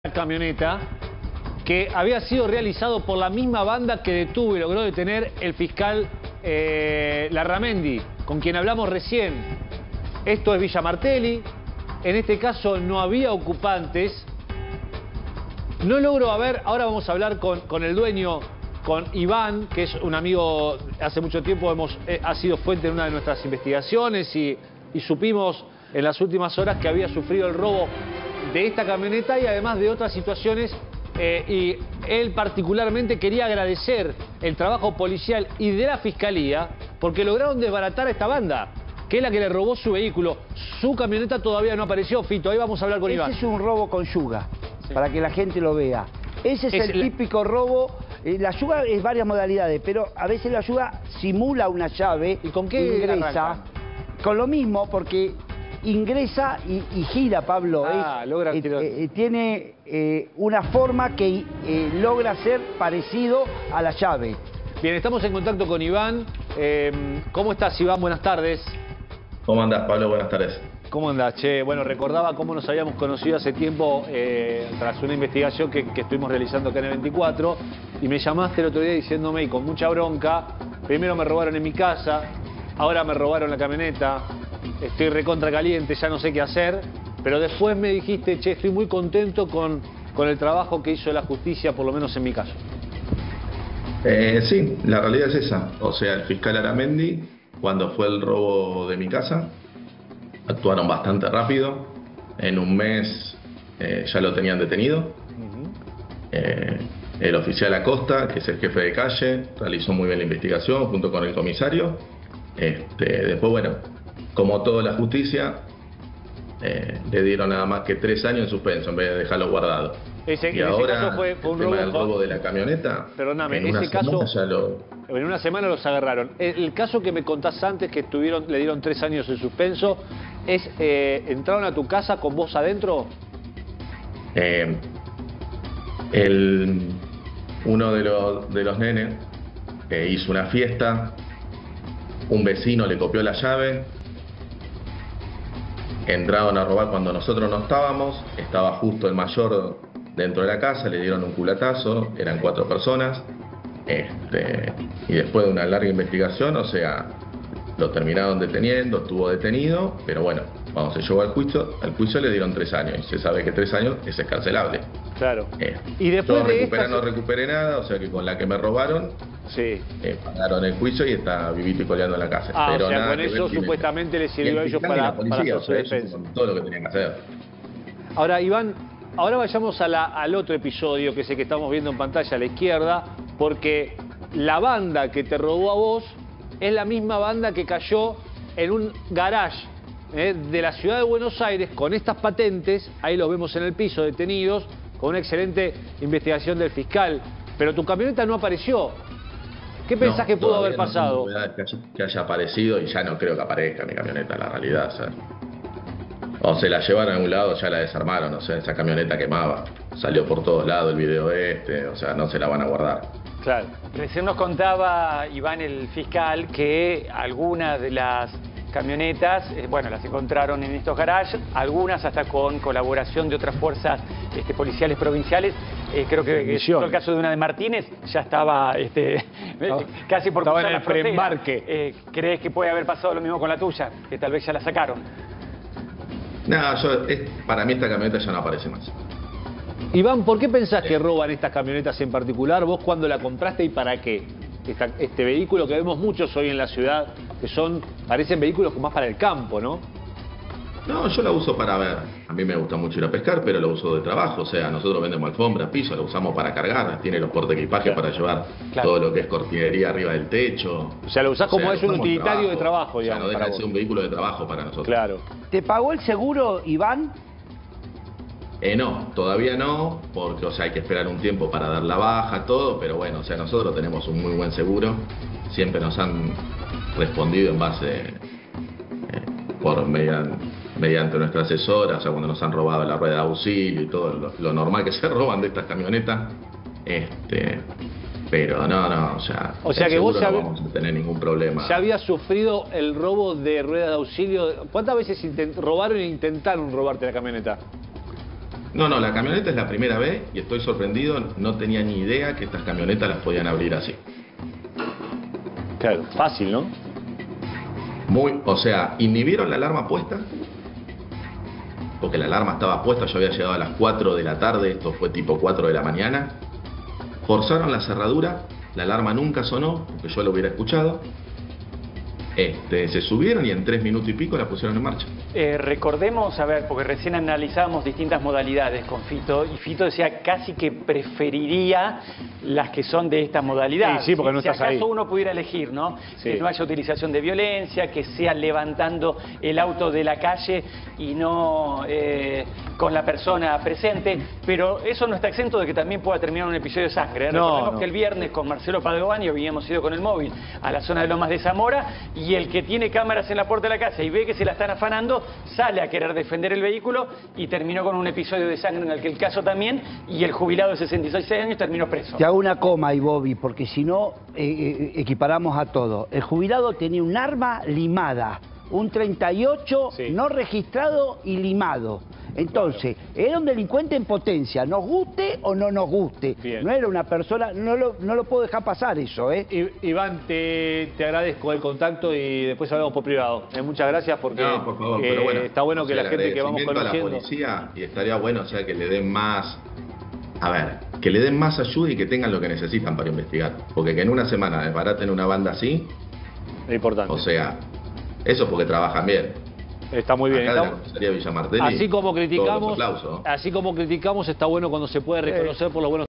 🚨 Cayó la banda que robaba camionetas de lujo
👉 Crece el robo de autos y camionetas. Hablamos con Iván, víctima del robo.

👉 Seguí en QuienCuandoDonde